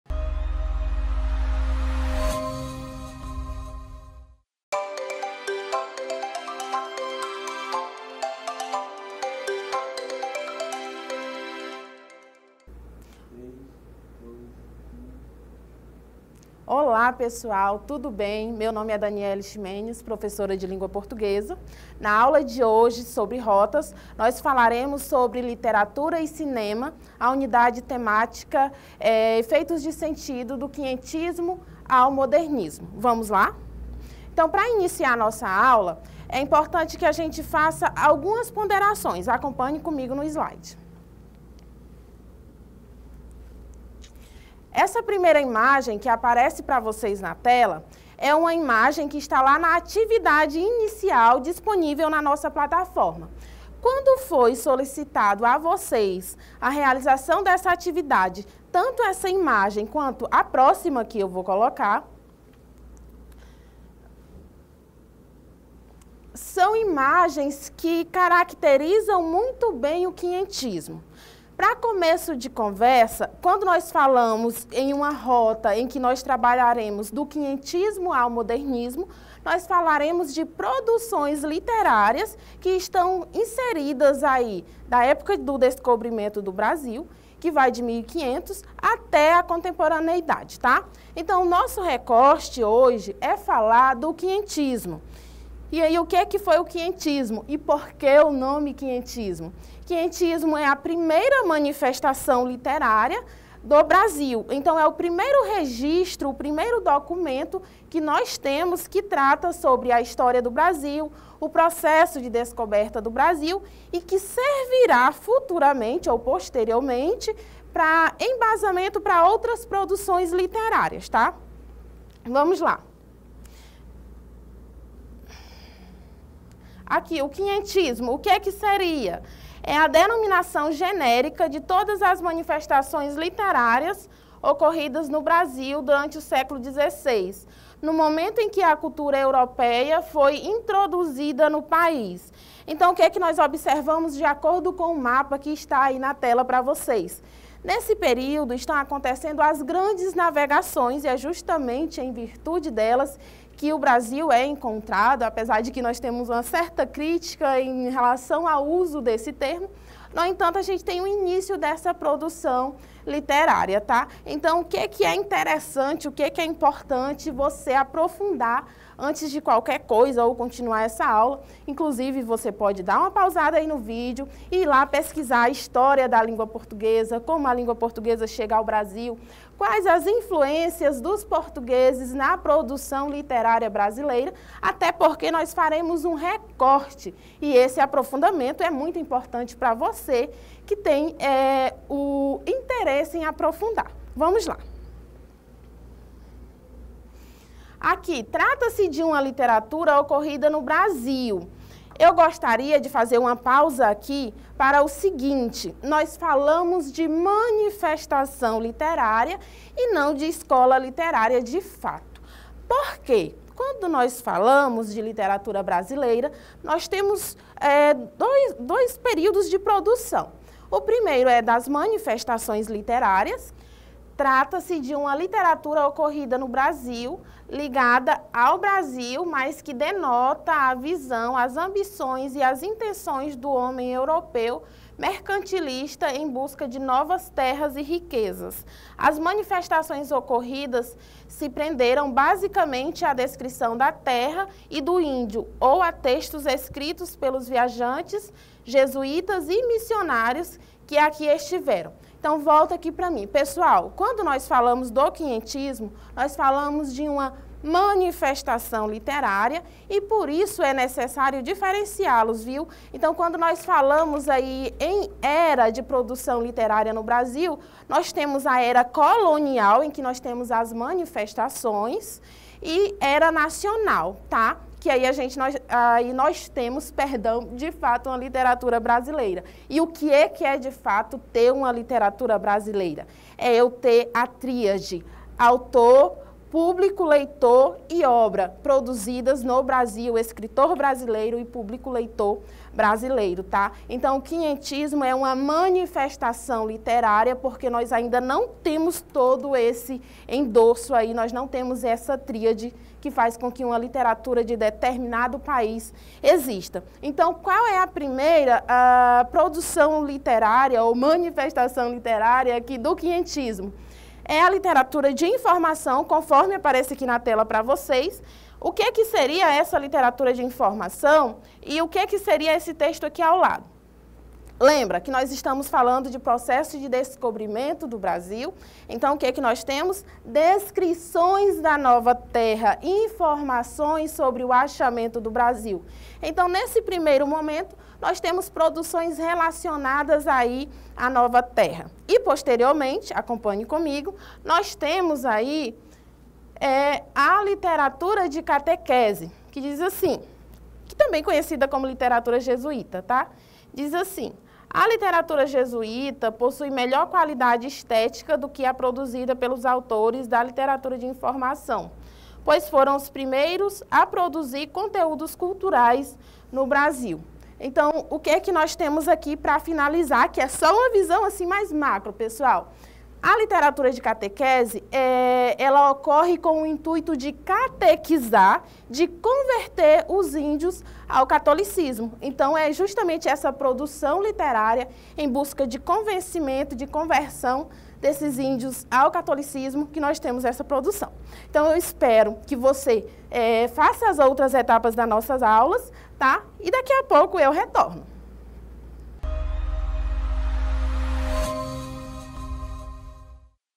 Educ. Híbrida- L.Portuguesa- Literatura e Cinema: A Memória Do Quinhentismo ao Modernismo-P.1/Aula 1
Educação Híbrida
Rota de Aprofundamento de Língua Portuguesa
Aula: Literatura e Cinema - A Memória Do Quinhentismo ao Modernismo Parte 1 - Aula 01
Professora:  Danielly Ximenes